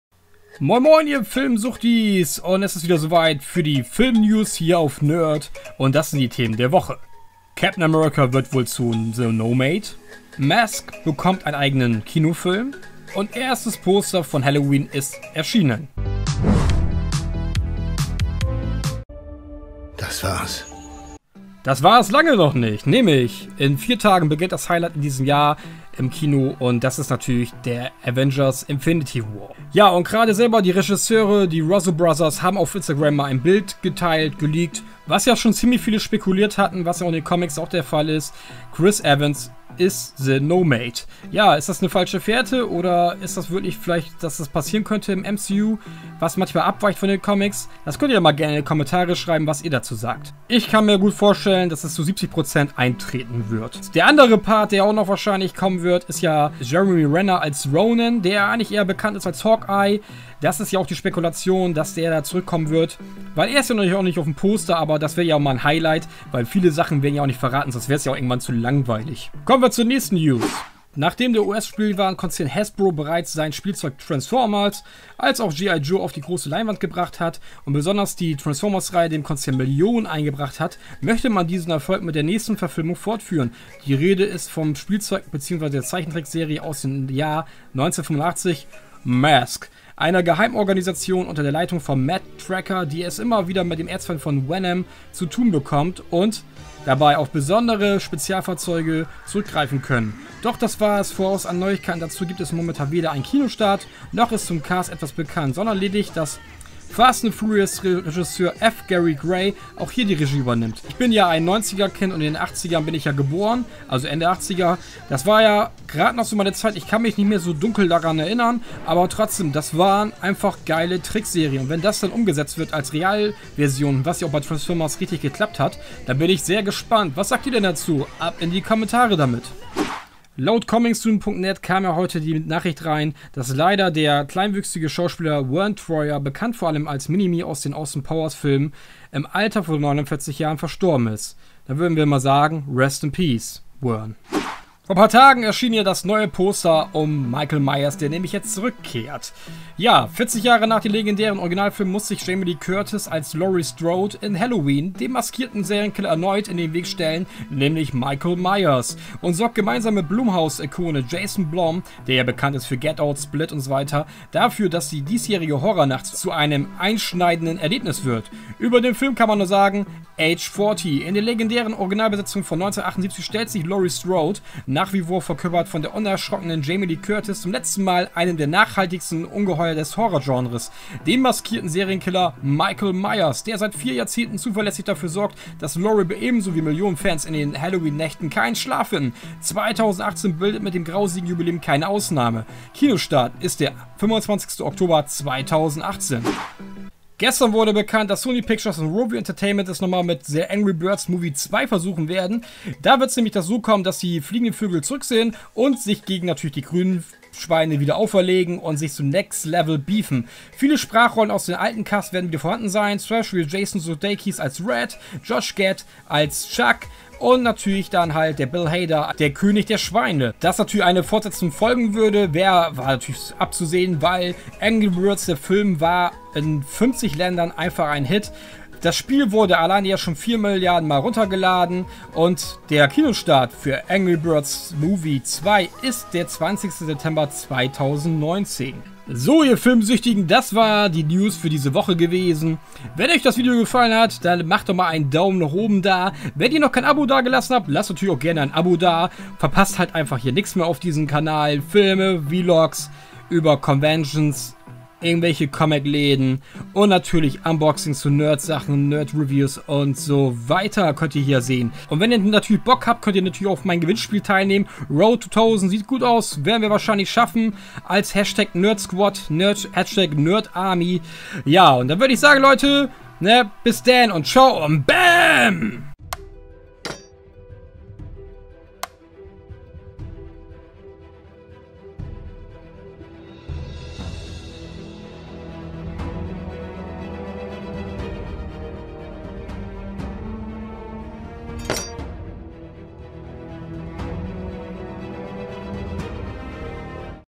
Captain America ist / wird NOMAD | Avengers Infinity War | NerdT News
Film News in Deutsch/ German - Captain America / Infinity War / Avengers Infinity War Trailer / Iron Man Hier kommen die neuen Film News über Avengers: Infinity War - Chris Evans/ Steve Rogers als Nomad, Hawkeye wird Ronin, Angry Birds 2 wird 2019 von Sony und Rovio ins Kino kommen. Mini-Me Darsteller Verne Troyer ist gestorben / dead / RIP. M.A.S.K Film kommt in die Kinos.

Quellen: comingsoon.net; Google; Instagram The Russo Brothers; Diverse Filmverleiher mit Pressezugang